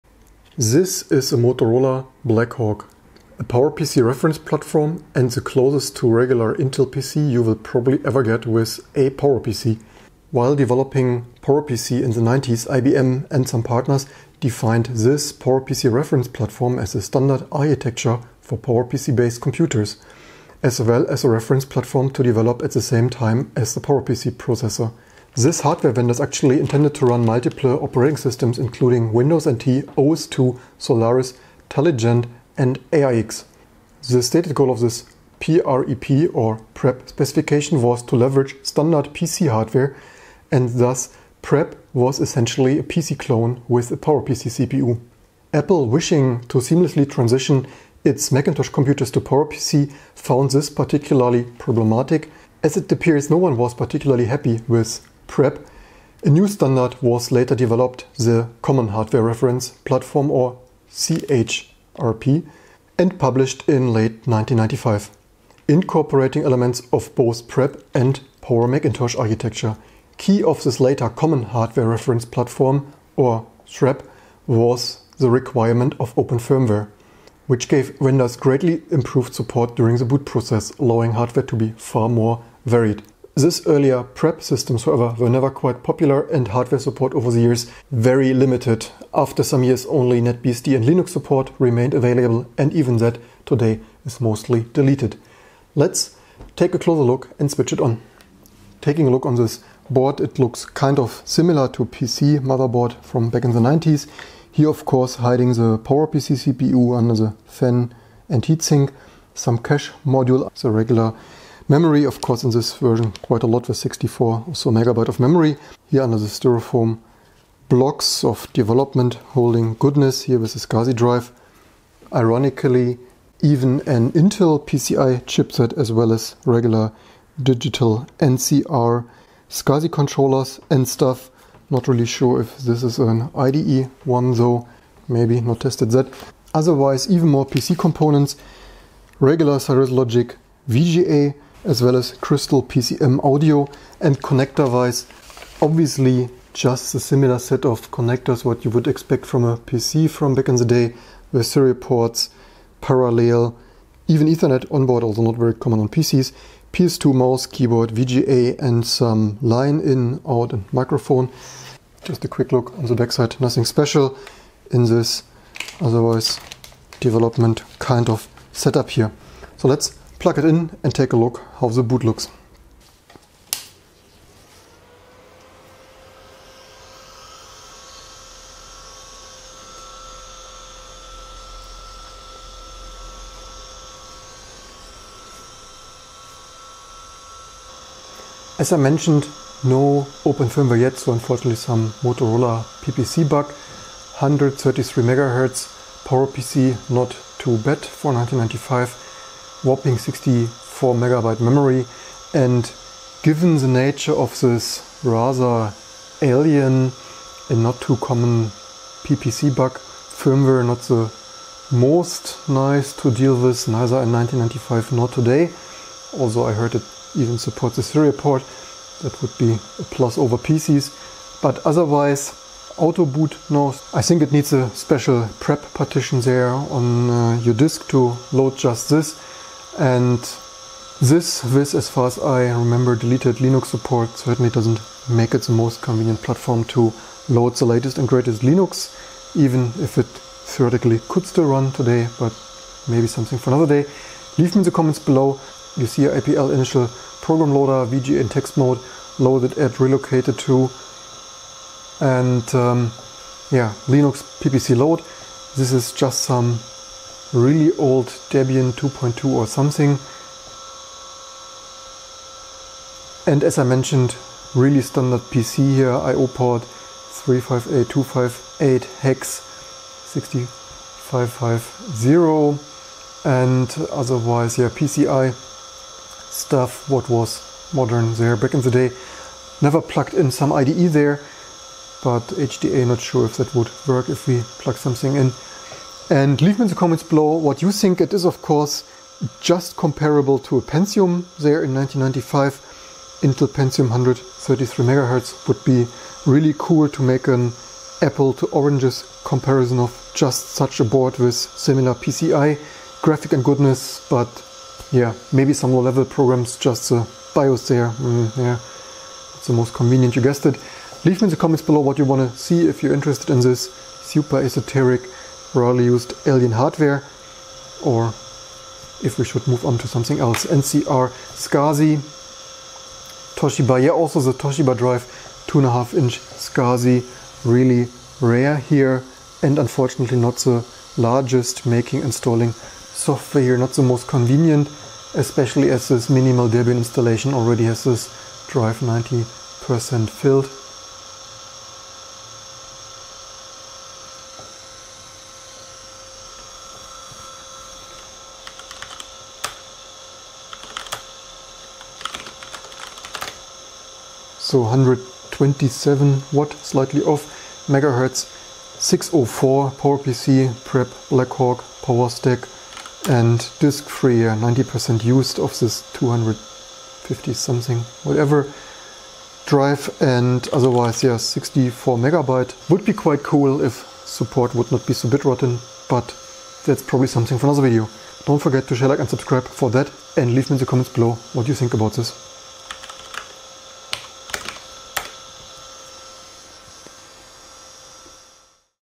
Motorola Blackhawk: a PC clone with a PowerPC 604 CPU from 1995!
PowerPC Reference Platform (PReP) was a standard system architecture for PowerPC-based computer systems (as well as a reference implementation) developed at the same time as the PowerPC processor architecture. Published by IBM in 1994.